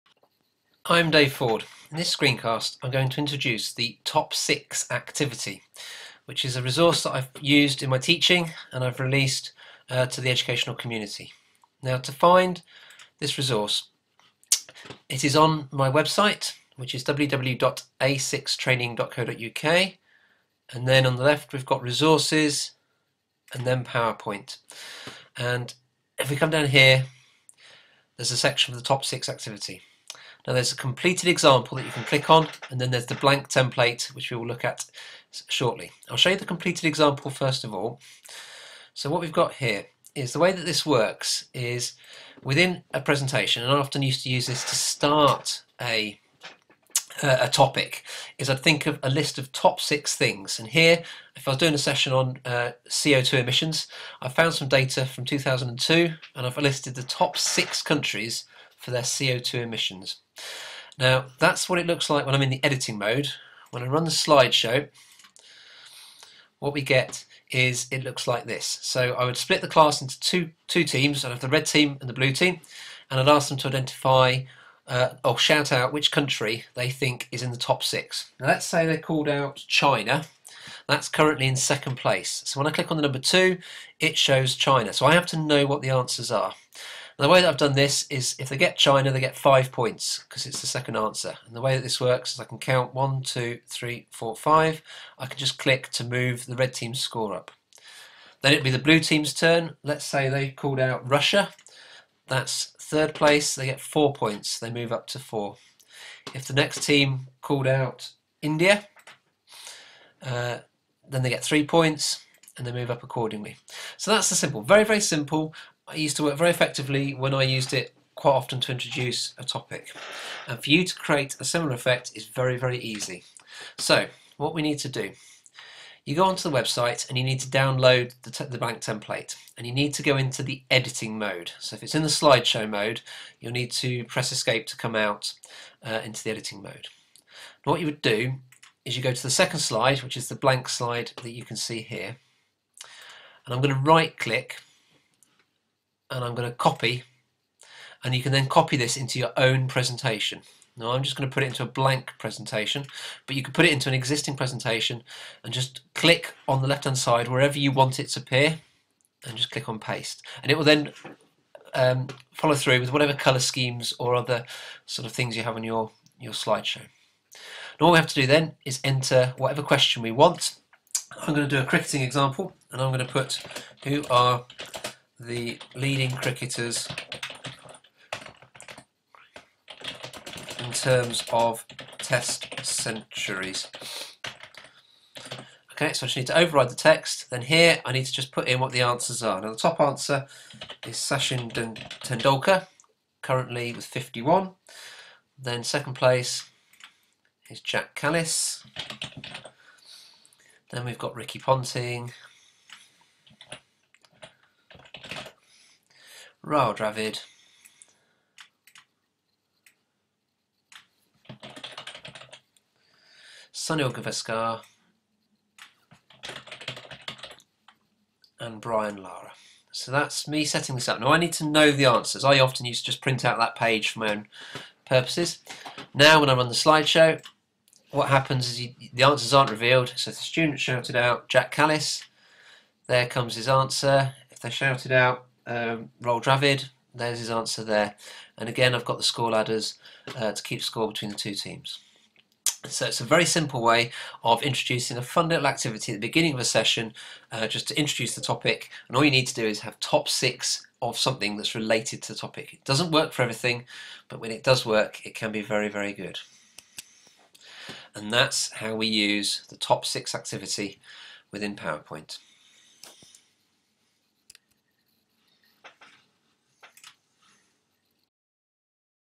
Screencast introducing the 'Top 6 Activity' for PowerPoint
Screencast introducing the 'Top 6 Activity' for PowerPoint A free to use template that can be copied and pasted into ones own PowerPoint presentation.